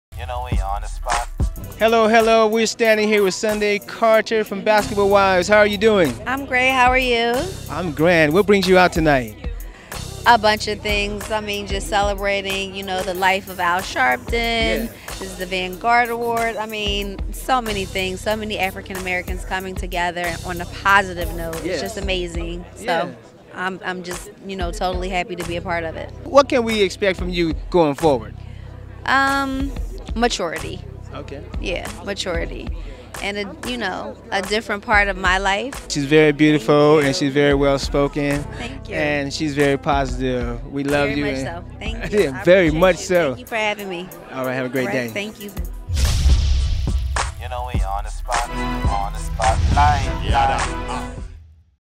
Sundy Carter - Basketball Wives - On Da Spot LIVE
Sundy Carter of Basketball Wives Season 3 interviewed by Guest Host Stevie Dreama of On Da Spot LIVE on the Red Carpet at Al Sharpton's 60th Birthday Celebration.

Sundy Carter is a Philadelphia native in her early 20s, who began her acting career in 1997. She studied at the prestigious Freedom Theatre and also attended Temple University where she majored in drama.

Sundy Carter's credits include numerous community projects, music videos with Method Man/Red Man, Tamar Braxton, Musiq Soulchild and Busta Rhymes.

Sundy Carter has also appeared on two episodes of the Jamie Foxx show and was featured on _Mothers Day (2002)_ on Lifetime TV with 'Latanya Richardson'.

Sunday Carter is also costarring alongside Beanie Sigel, Jay-Z and Damon Dash in _State Property (2002)_. Sundy has modeled in several issues of Solid Gold magazine and Style Q.